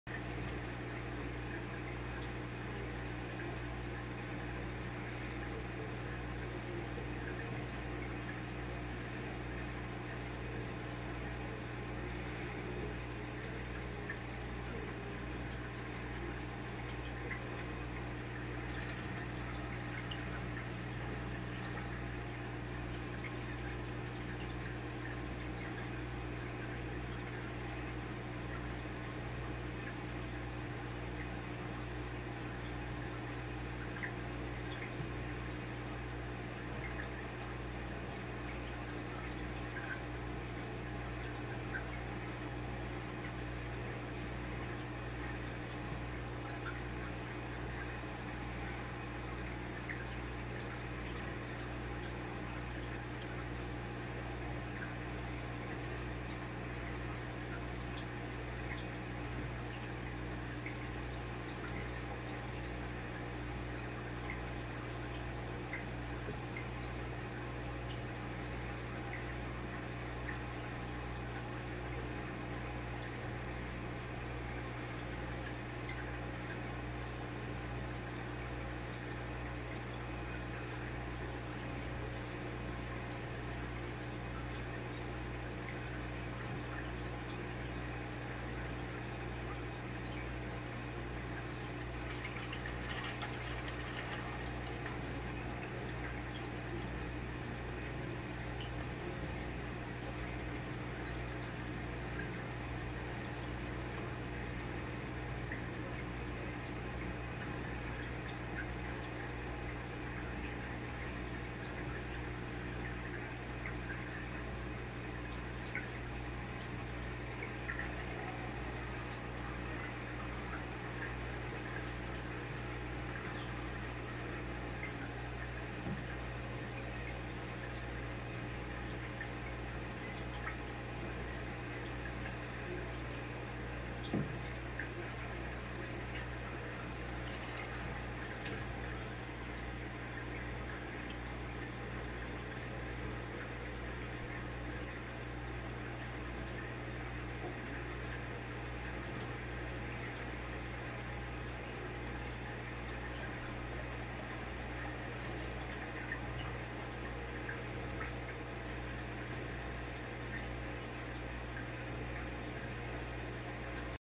Turtle (RES) Claws stuck in wave-maker @ the 2:05 mark.
Maci the turtle getting a bubble  Massage & claws getting stuck in wave-maker.  🐢 😬